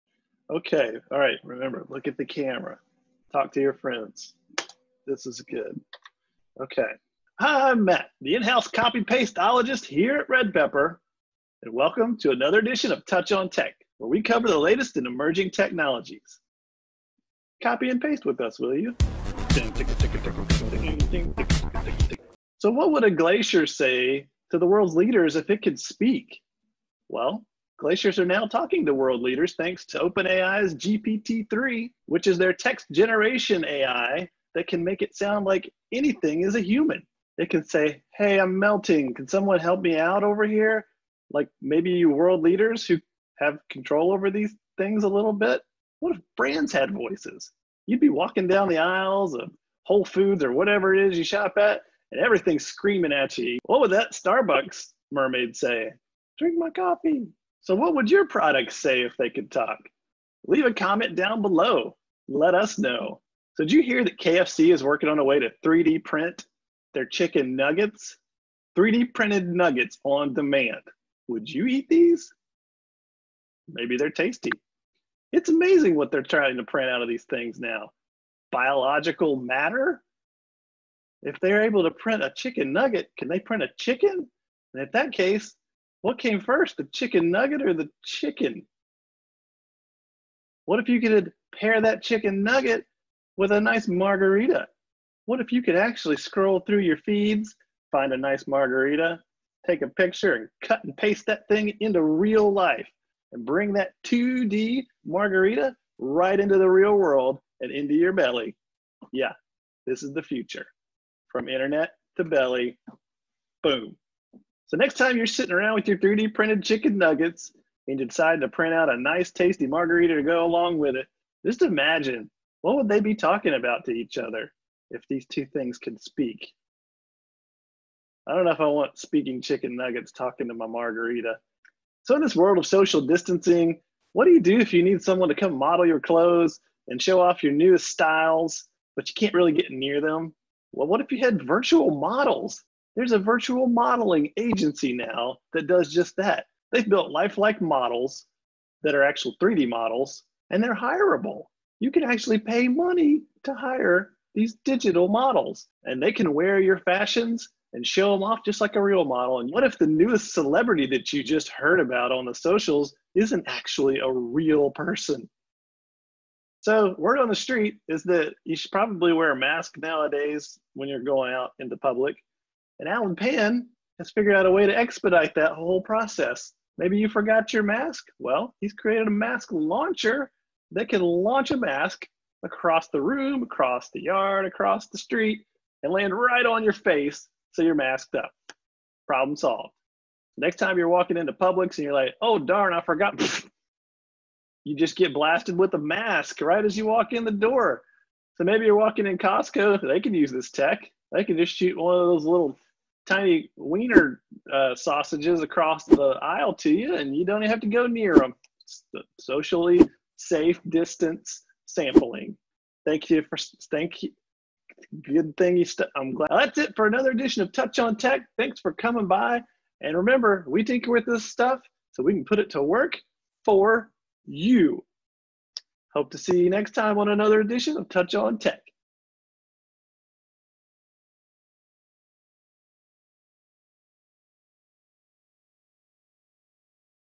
Touch on Tech Season 3 Episode 12- Copypaste-ologist
What did the glacier say to the world leader? Well, according to OpenAI’s GPT-3 model, it would say, “We are melting. It hurts.” GPT-3 wrote climate change protest letters to Trump, Xi, and Putin. And if all inanimate objects could talk? That'd be one loud trip to the grocery store.

Hungry? Just 3D print yourself some chicken nuggets. Ew. But also—we’re listening. KFC is partnering with Moscow firm, 3D Bioprinting Solutions to print these nuggies. They’re more environmentally friendly. But are they tastebud friendly? TBD.

In other strange news, a photographer introduced a virtual modeling agency with lifelike 3D digital models you can hire for your next photoshoot. These virtual humans are made on tools like Blender, an open source 3D modeling software. At least it helps keep people safe?

A fellow named Allen Pan made a mask launcher. It’s kind of like a net launcher, but it catches faces. So if you ever forget your mask, be on the lookout one flying through the air.